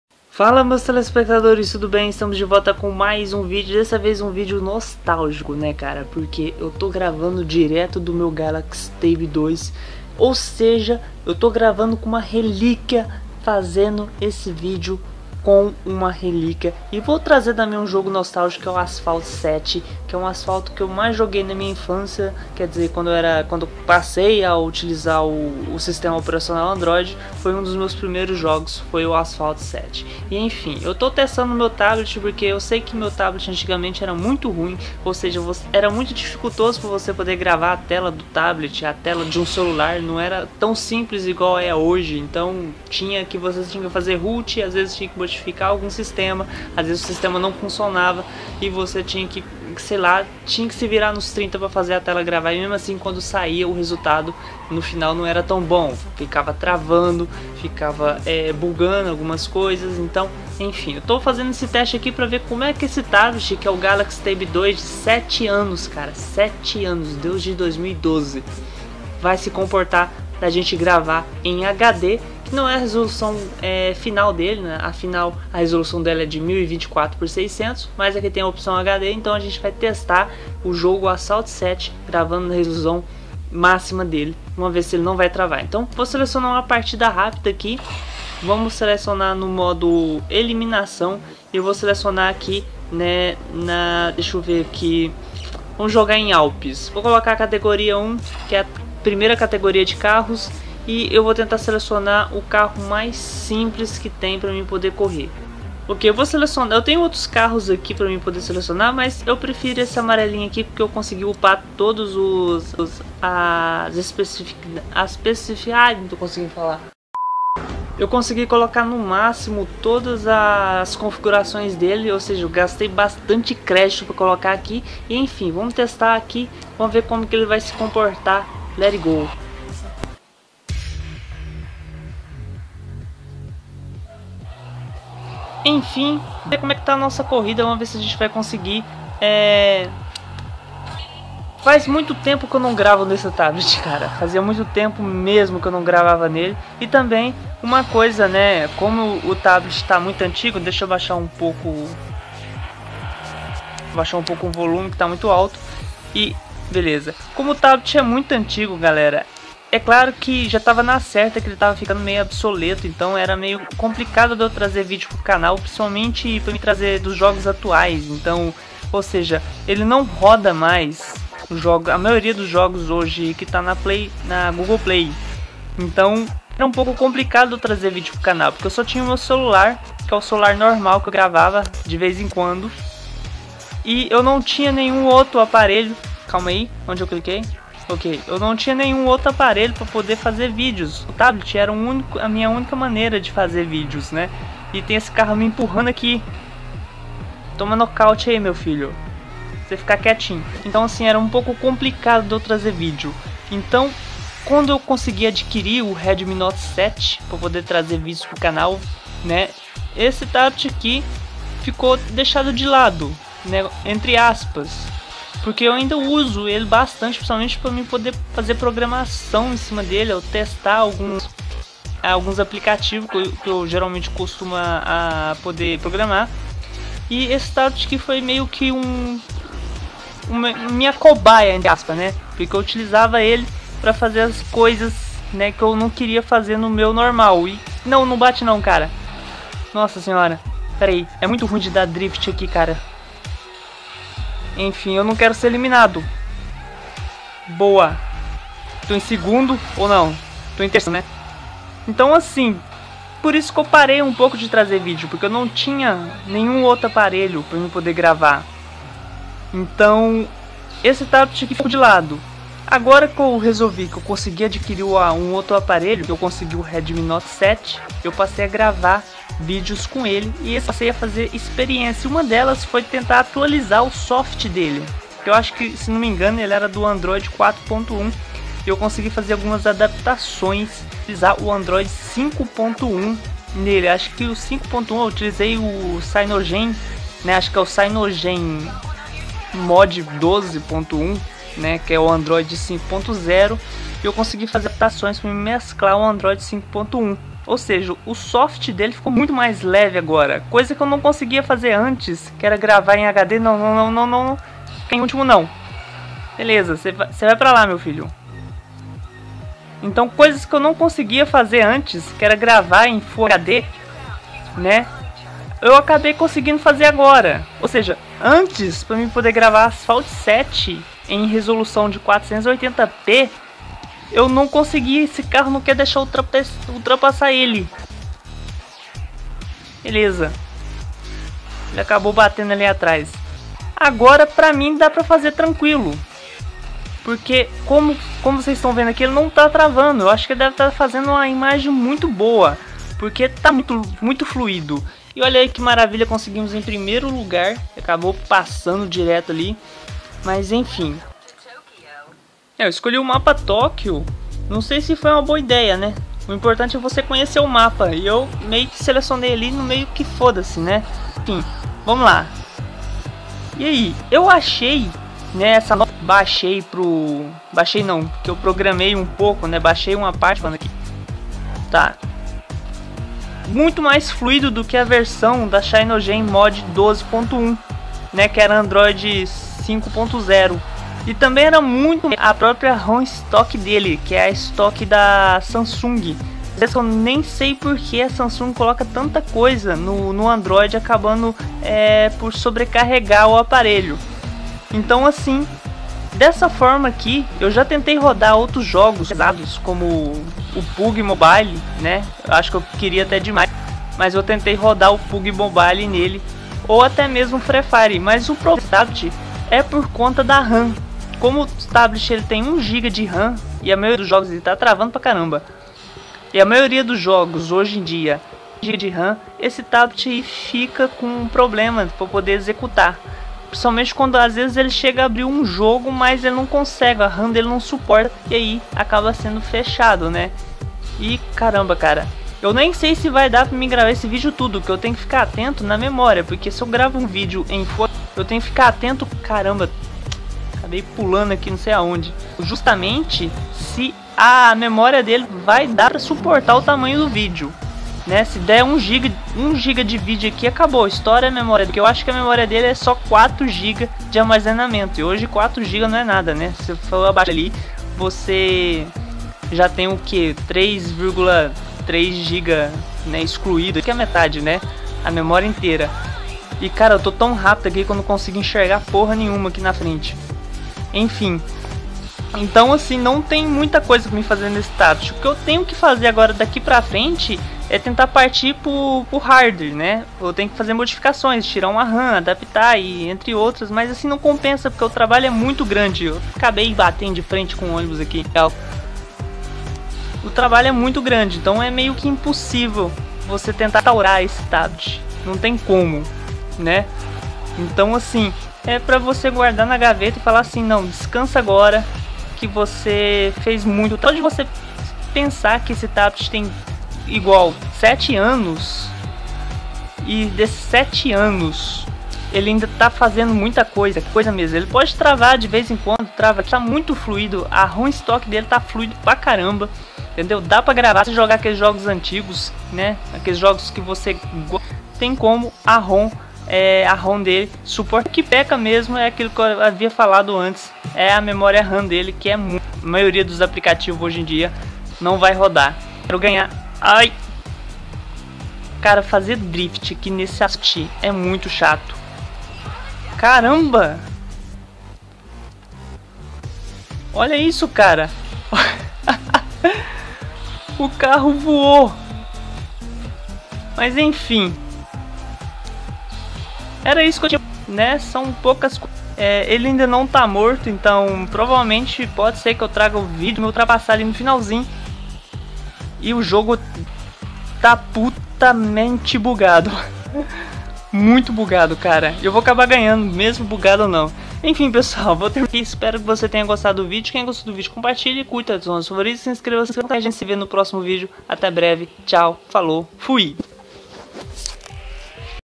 Ressuscitando o Galaxy Tab 2
Oie, meus inscrito. Trago à vocês uma pequena gameplay retrazendo novamente o meu primeiro tablet que foi usado para gravar vídeos por anos e dar a tona a este incrível canal. Espero que vocês gostam!

ESPECIFICAÇÕES SOBRE O APARELHO

Modelo: Galaxy Tab 2 P3110
Capacidade de Armazenamento: 16 GB
CPU: 1,0 GHz dual core (internacional) CORTEX A9
GPU: PowerVR SGX540
Resolução da tela: 1024x600

Siga as séries do canal.

Siga as videos aulas do canal